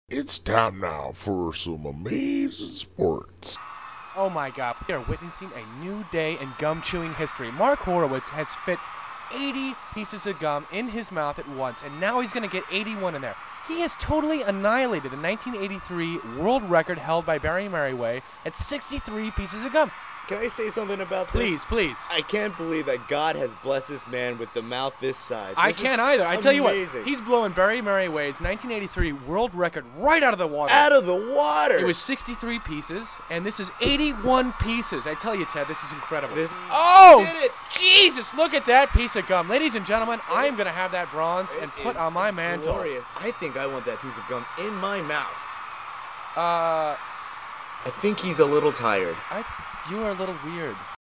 25th Annual Sample Gum Chew-off w/ commentary
Here is the beloved gum chewing competition with real live sportscaster's commentary.